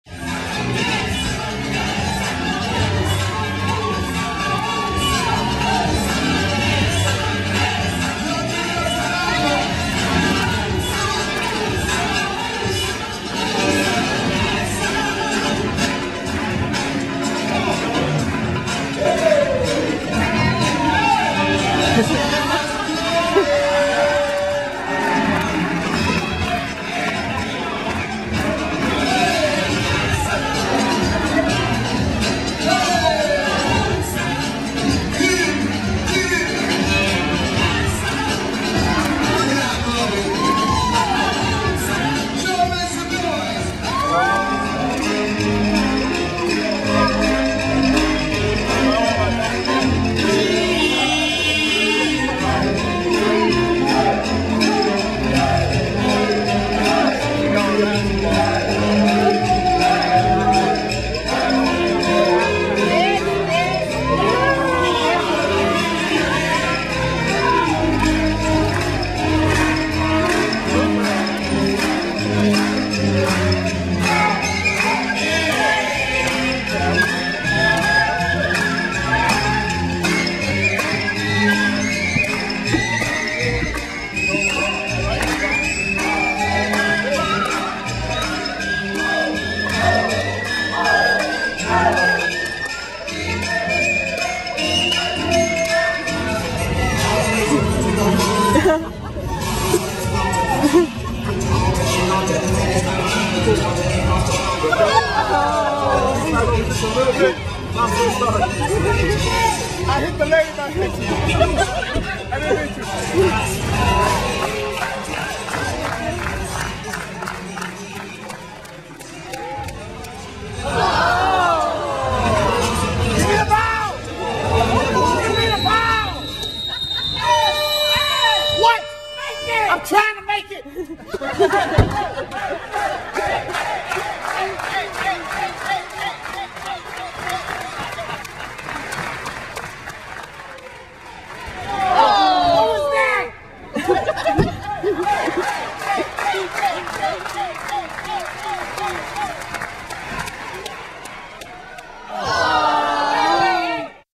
Harlem Globetrotters Pregame 1/10/14
The Harlem Globetrotters returned to Morris, MN for a night of fun entertainment.